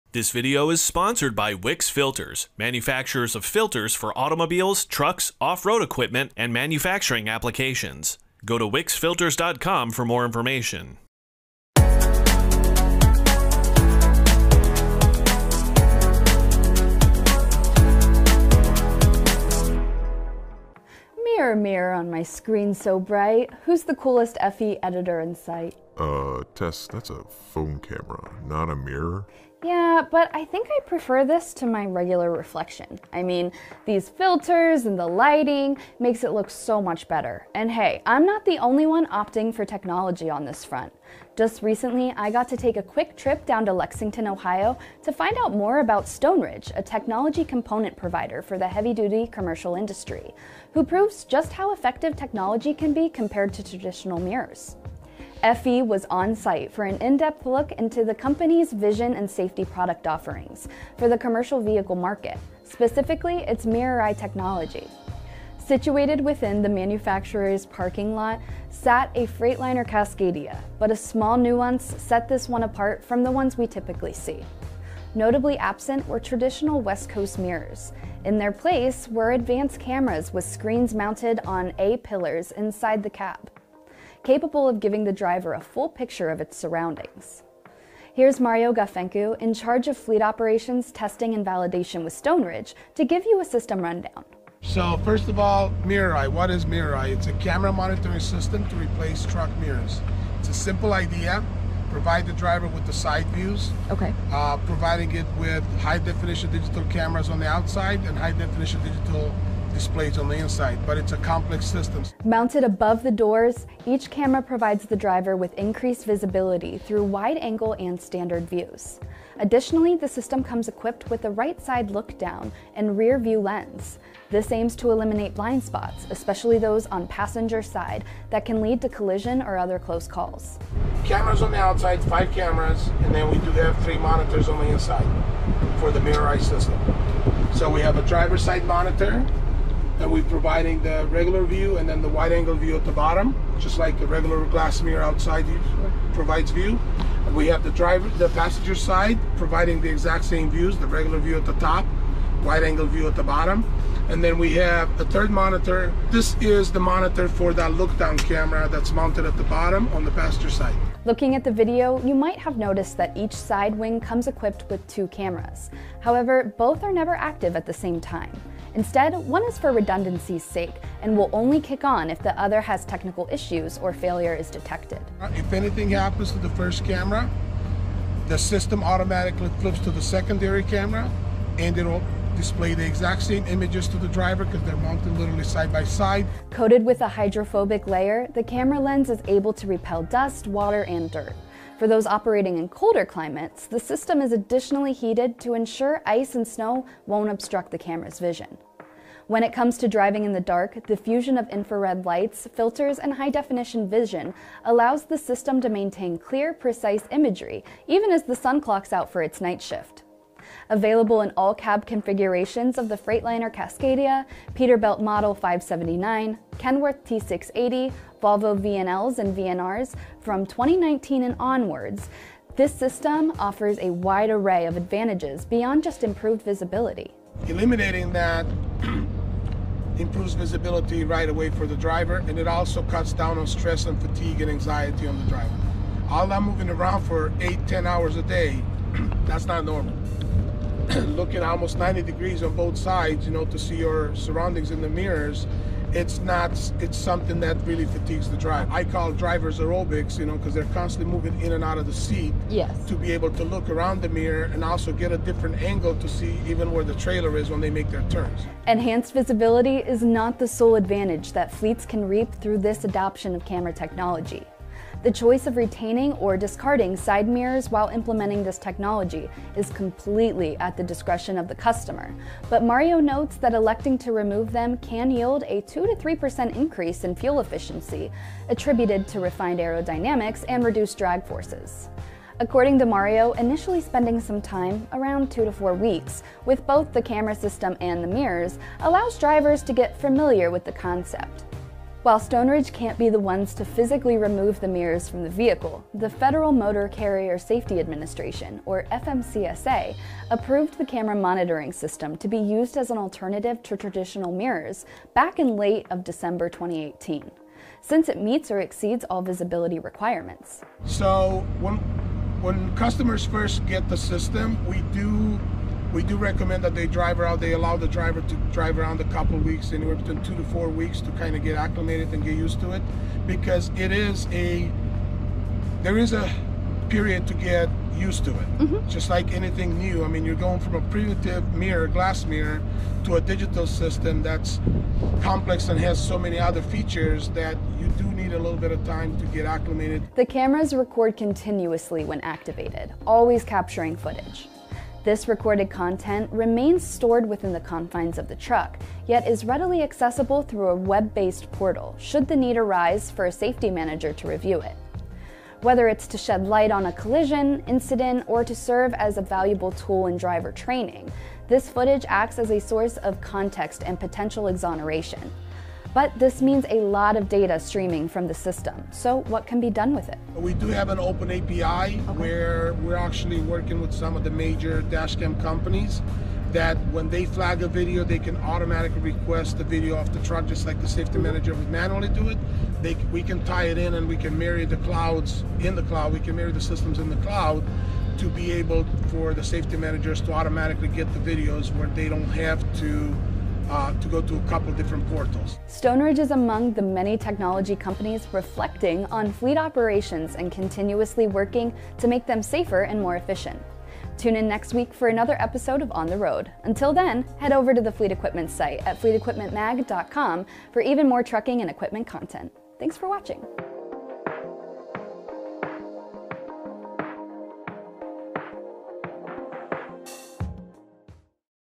Seeing beyond mirrors with truck camera vision systems | FE On The Road
Fleet Equipment was on-site for an in-depth exploration of the company's vision and safety product offerings for the commercial vehicle market, specifically its MirrorEye technology. In the manufacturing plant's parking lot, a Freightliner Cascadia was parked, distinguished from others by a notable absence: traditional west coast mirrors. Instead, advanced cameras with screens mounted on the A-pillars inside the cab provide drivers a comprehensive view of their surroundings.

Mario Gafencu, responsible for fleet operations, testing and validation with Stoneridge, offered a system rundown.

"It's a camera monitoring system to replace truck mirrors. The idea is simple: provide side views for the driver using high-definition digital cameras on the outside and high-definition digital displays on the inside. However, it's a complex system," he said.

Installed above the doors, the cameras grant drivers enhanced visibility through wide-angle and standard views. The system is also equipped with right-side lookdown and rear view lenses designed to eradicate blind spots, particularly on the passenger side, which can lead to near misses or collisions.

"We have five cameras on the outside and three monitors inside for the MirrorEye system. The driver's side monitor provides the regular view with a wide-angle view at the bottom, mirroring the functionality of a traditional glass mirror," Gafencu explained. "The passenger side monitor provides identical views. We also have a third monitor for the look-down camera mounted at the bottom on the passenger side."

Upon inspecting the video, viewers might observe that each side wing is equipped with two cameras. However, both are not active simultaneously. One serves as a backup, activating only if its counterpart encounters technical difficulties.

"If anything happens to the first camera, the system switches to the secondary camera. It will display identical images to the driver since they're mounted side by side," Gafencu added.

A hydrophobic layer coats the camera lens, repelling dust, water, and dirt. To cater to those in colder regions, the system is heated to prevent ice and snow from blocking the camera’s view.

For nighttime driving, the combination of infrared lights, filters, and high-definition vision ensures the system offers sharp, accurate imagery even after sunset. Compatible with all cab configurations of the Freightliner Cascadia, Peterbilt Model 579, Kenworth T680, Volvo VNLs, and VNRs from 2019 onward, this system brings numerous benefits beyond improved visibility.

"Doing away with mirrors immediately enhances visibility," Gafencu said. "It also reduces the driver's stress, fatigue, and anxiety. Moving around for eight to 10 hours a day isn't typical. The need to constantly check mirrors places a strain on the driver."

Describing the challenges, he added, "It's like drivers are doing aerobics. They frequently adjust their positions to get a better view around the mirror, especially when making turns."

Enhanced visibility isn't the only perk. While deciding to retain or discard side mirrors during this technology's implementation is up to the customer, Gafencu mentioned that removing them could result in a 2-3% fuel efficiency increase due to better aerodynamics and reduced drag.

Initially, spending a few weeks with both the camera system and mirrors helps drivers acclimate, Gafencu advised.

Although Stoneridge isn't responsible for physically removing mirrors, the Federal Motor Carrier Safety Administration (FMCSA) approved the camera monitoring system as an alternative to traditional mirrors in December 2018, as it meets or surpasses all visibility criteria.

Gafencu elaborated, "When customers first get the system, we suggest letting drivers familiarize themselves for two to four weeks. Transitioning from a basic glass mirror to a sophisticated digital system requires some adjustment."

The cameras record continuously when active. This data, stored in the truck, is easily accessible via a web portal if safety managers need to review it. The footage can clarify incidents or serve as a training tool for drivers. Considering the volume of data generated, what happens to it?

"We have an open API. We're collaborating with major dashcam companies. When they highlight a video, they can retrieve it from the truck in the same way a safety manager would," Gafencu said. "We can integrate our systems, allowing safety managers to retrieve videos without navigating multiple portals."